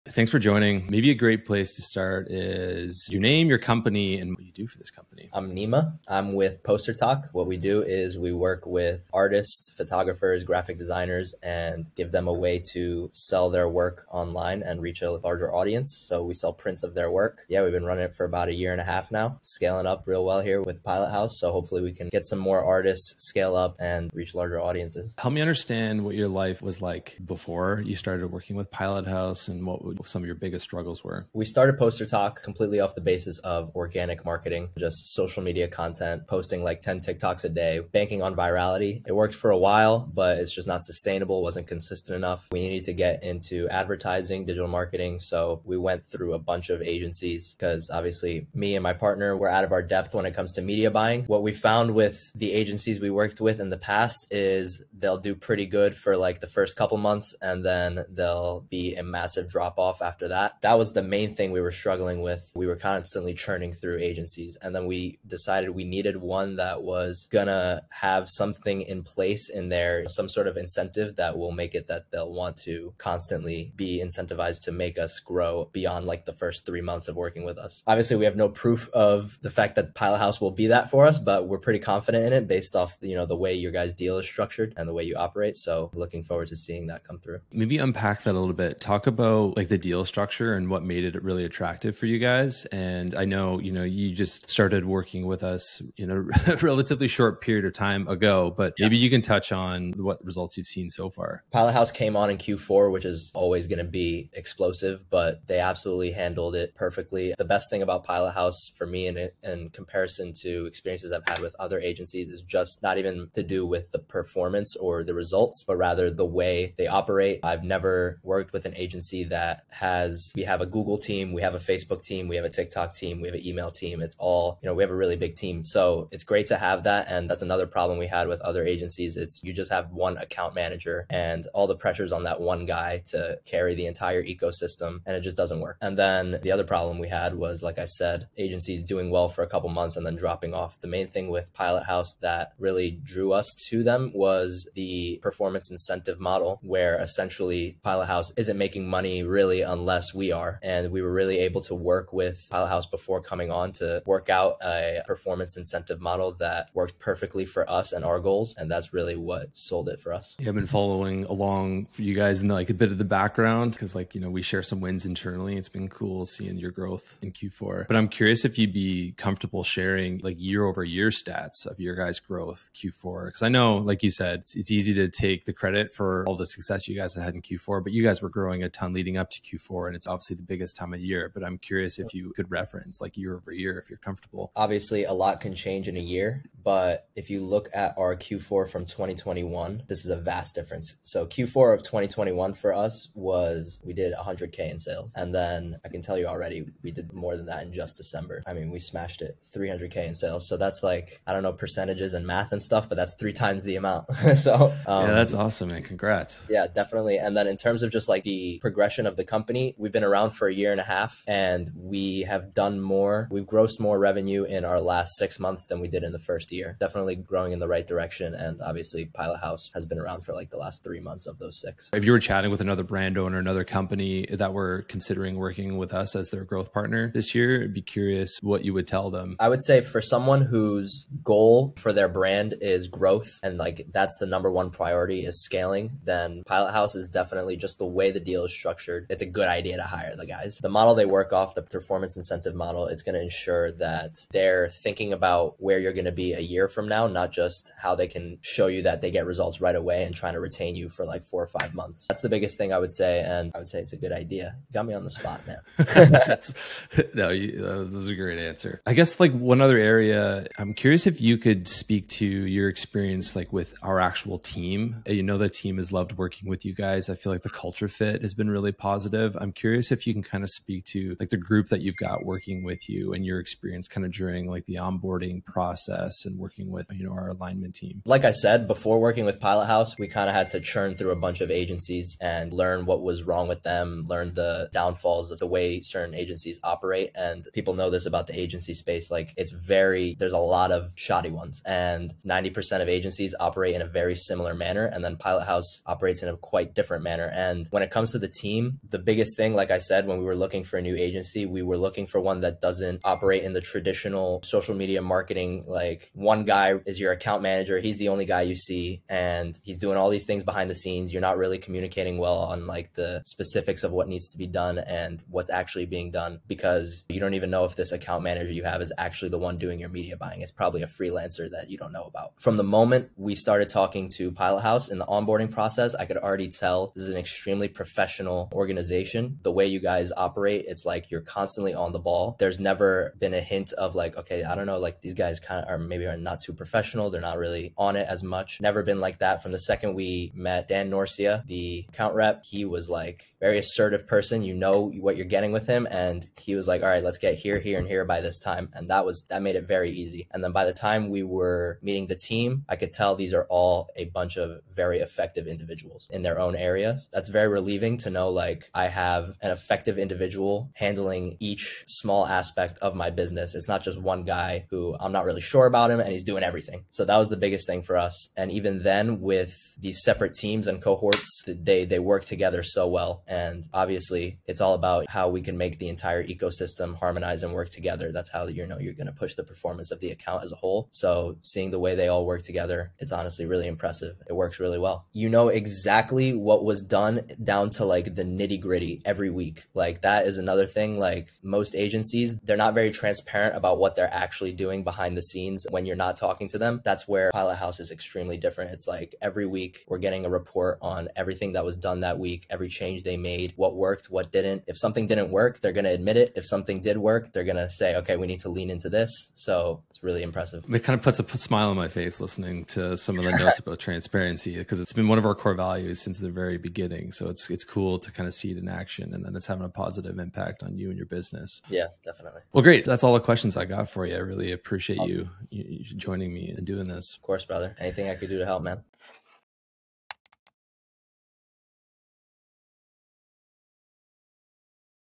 Postertok x Pilothouse
Since 2022, Pilothouse has worked with Postertok to improve the efficiency of its digital advertising. In Q4/2022, Posterok experienced 300%+ growth in revenue YoY and they are continuing to grow in 2023.

This is a conversation with Nima Moghaddasi (Co-founder) about their experience working with Pilothouse